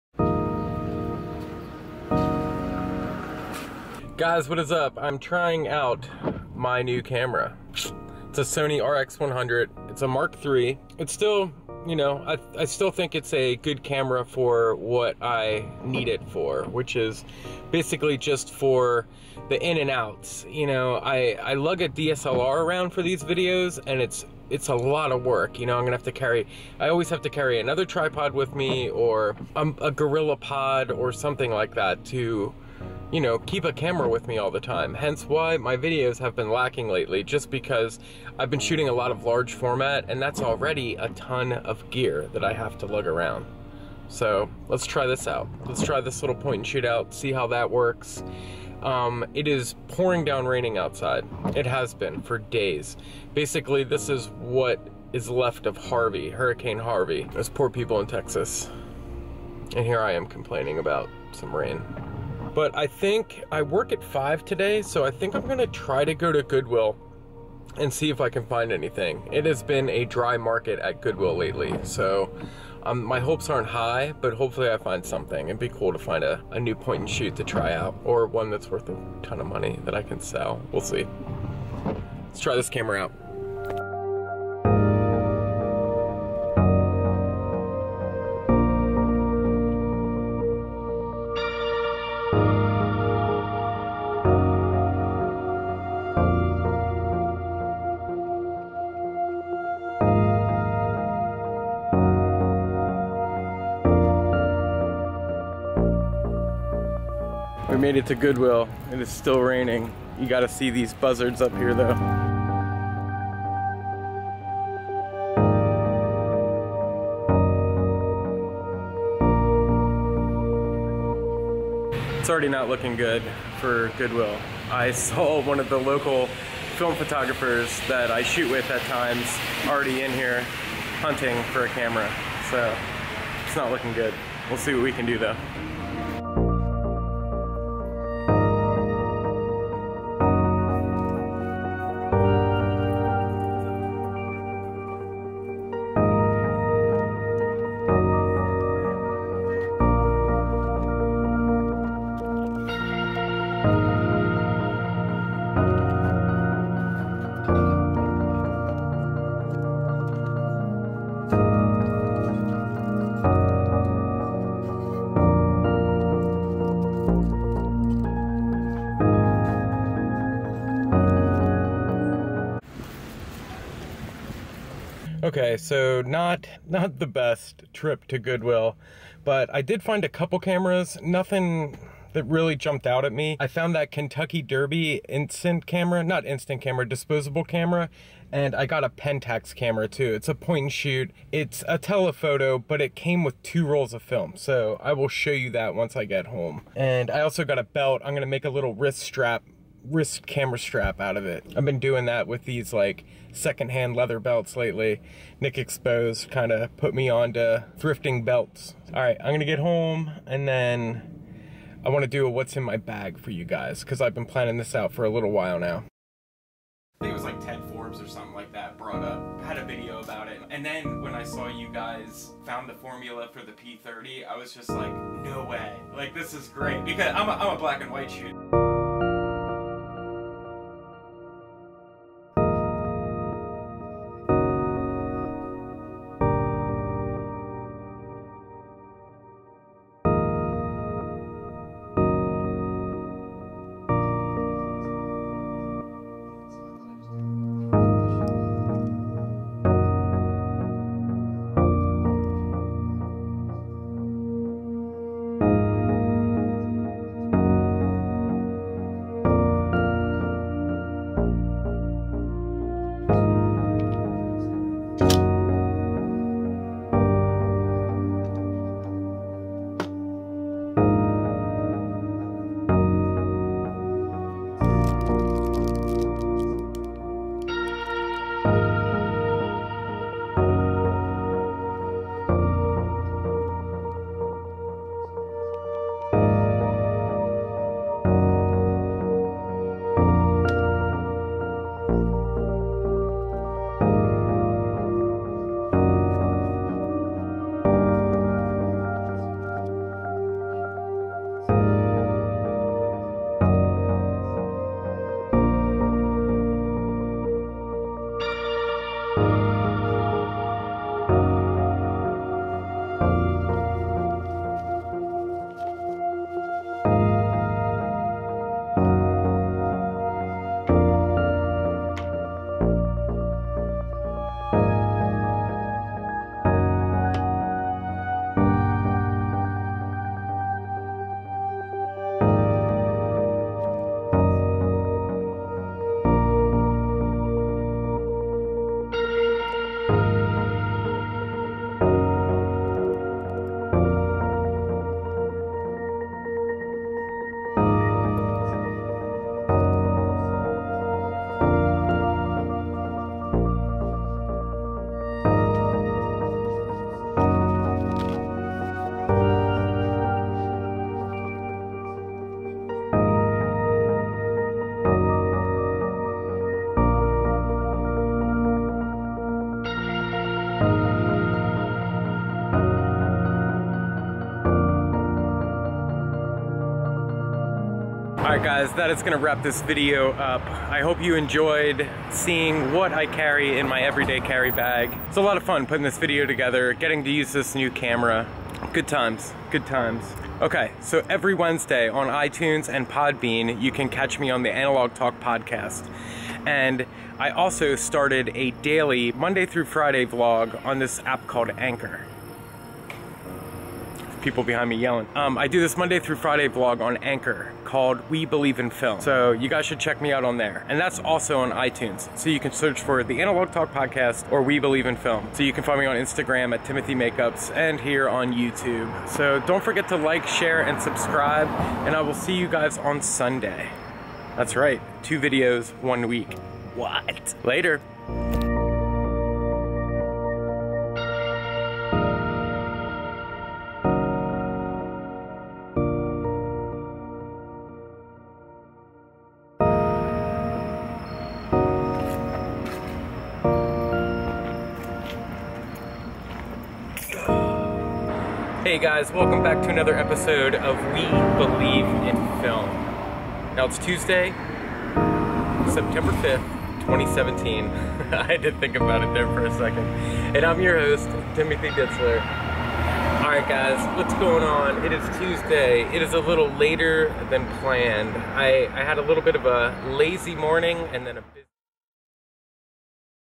What's in my bag??? (Analog Photography)
Skip to what's in my bag - 3:52

This episode kicks off my attempt at a biweekly video schedule! I'm really going to give it a go guys!
I take you with thrifting at my local Goodwill. Then we take a look what I carry with me in my everyday carry bag. Then we finish off with a little behind the scenes of both podcasts I host and Cohost.
I'll see you guys Sunday!!!
-Timothy